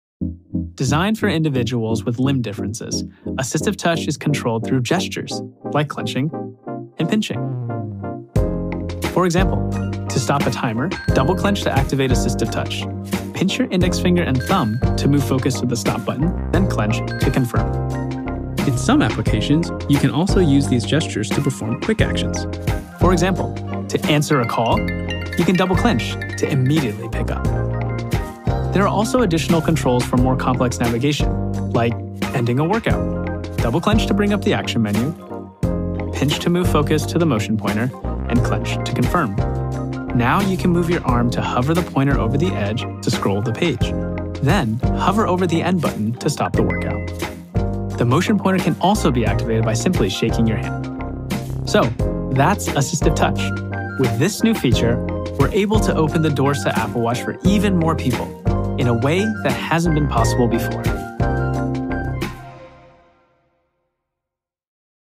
Introducing AssistiveTouch on Apple Watch [CC]
Introducing AssistiveTouch on Apple Watch

To support users with limited mobility, Apple is introducing a revolutionary new accessibility feature for Apple Watch. AssistiveTouch for watchOS allows users with upper body limb differences to enjoy the benefits of Apple Watch without ever having to touch the display or controls. Using built-in motion sensors like the gyroscope and accelerometer, along with the optical heart rate sensor and on-device machine learning, Apple Watch can detect subtle differences in muscle movement and tendon activity, which lets users navigate a cursor on the display through a series of hand gestures, like a pinch or a clench. AssistiveTouch on Apple Watch enables customers who have limb differences to more easily answer incoming calls, control an onscreen motion pointer, and access Notification Center, Control Center, and more.